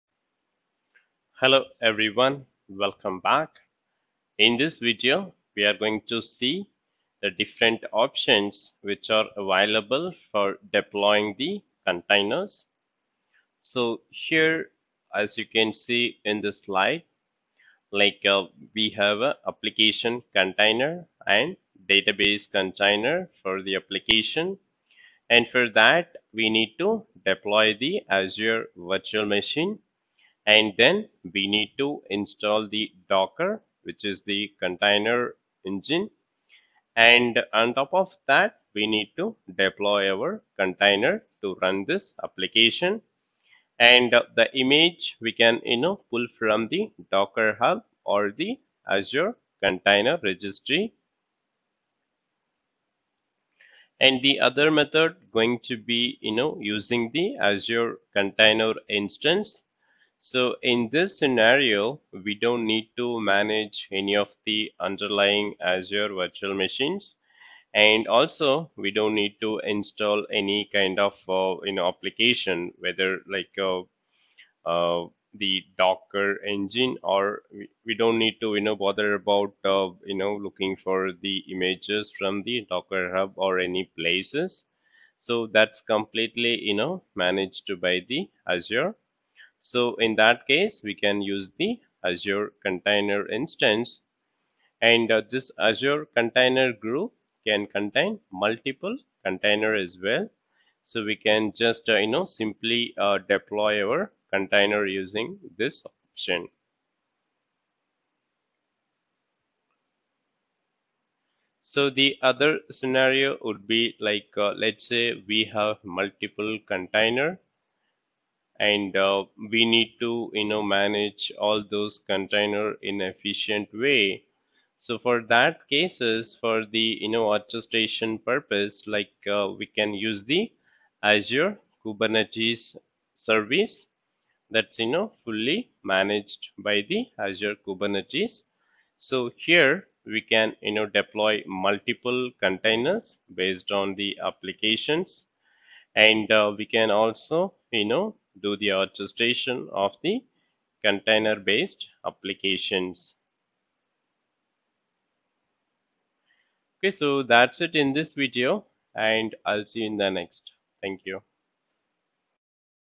7. AZ-305 - Container Deployment Options
Microsoft Azure Solutions Architect Expert course || AZ-305 course with Hands-On Lab || Module 01 - Design Infrastructure Solutions || Container Deployment Options ||